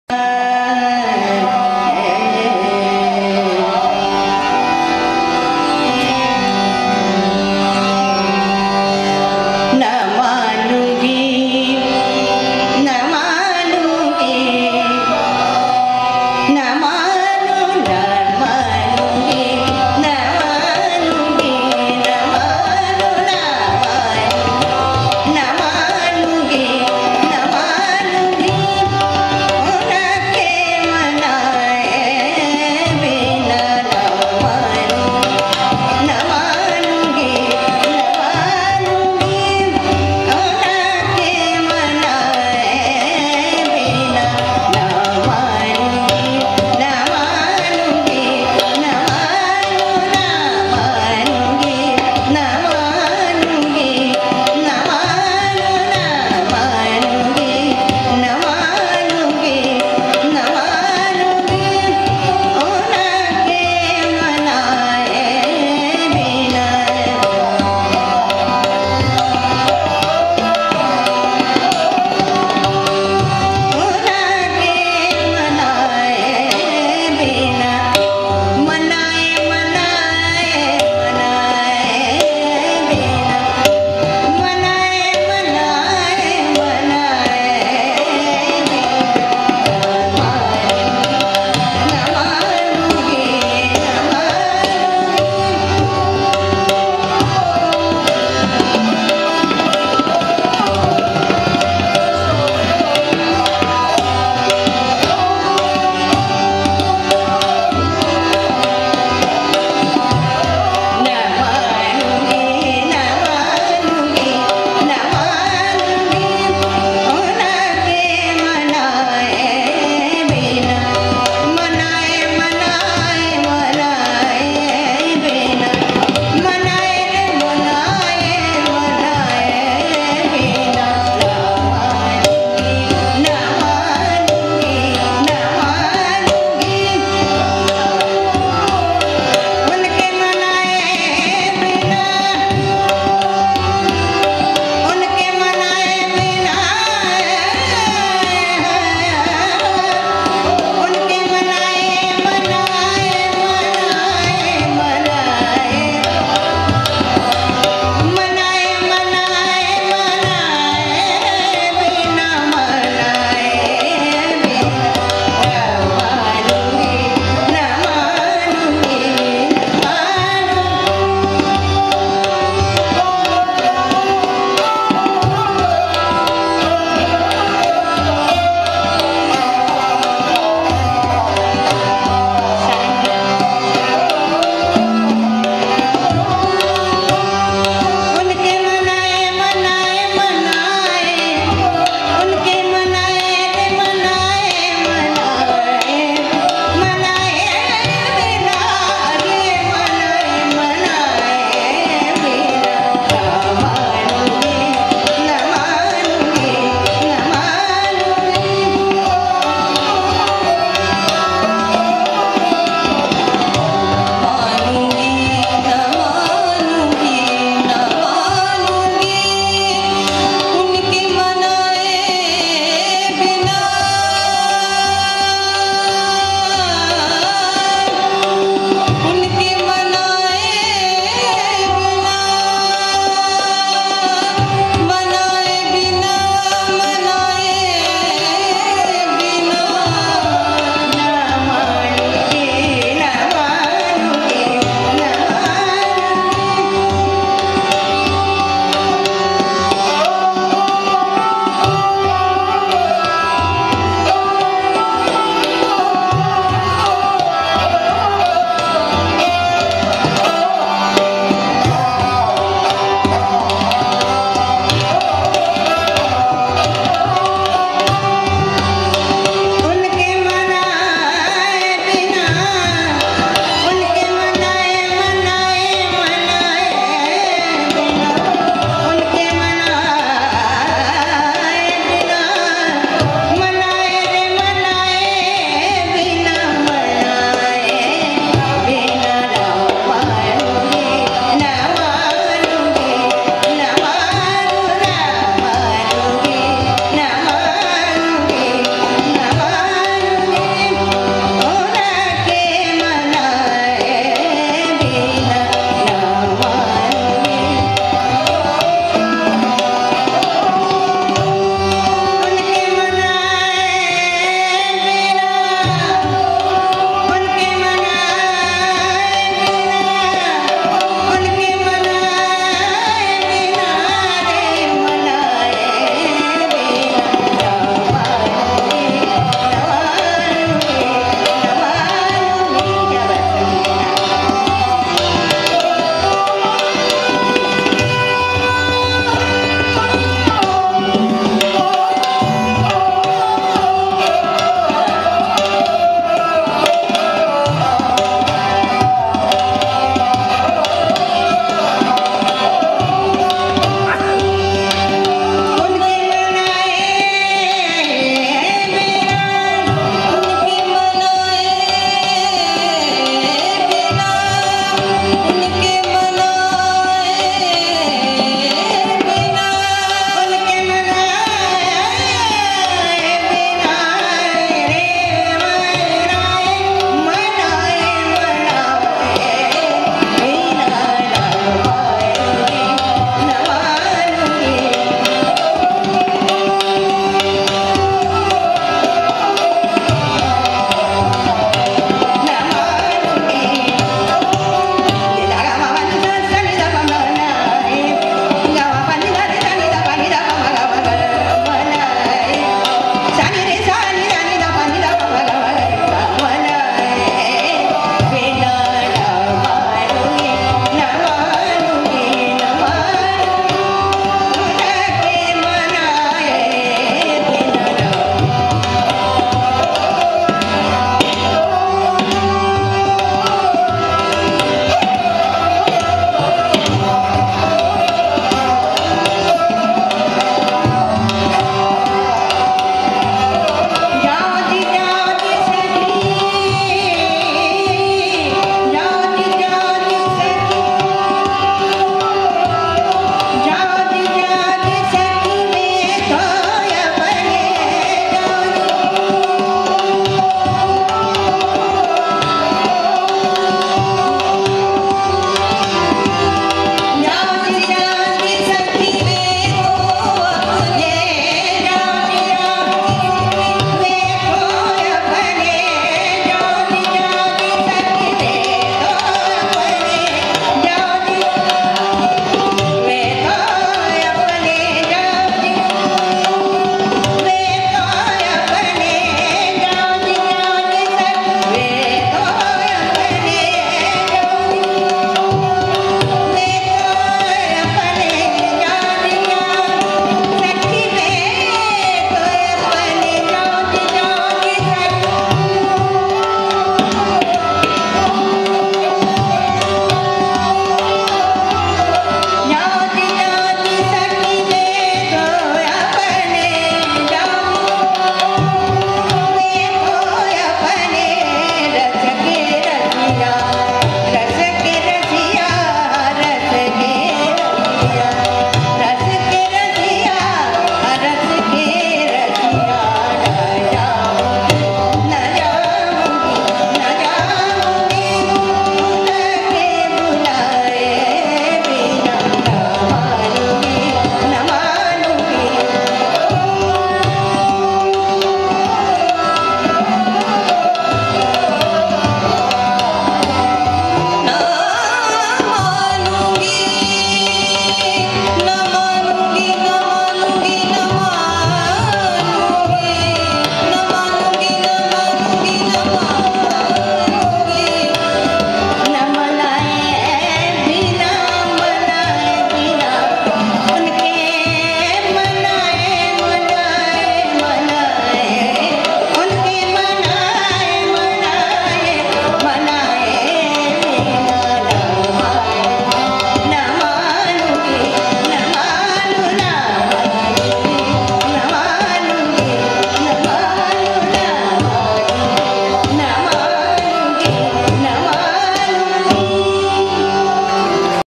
Vidhushi Smt. Purnima Choudhury and Ustad Sabir Khan
Vocal: Vidhushi Smt. Purnima Choudhury
Tabla: Ustad Sabir Khan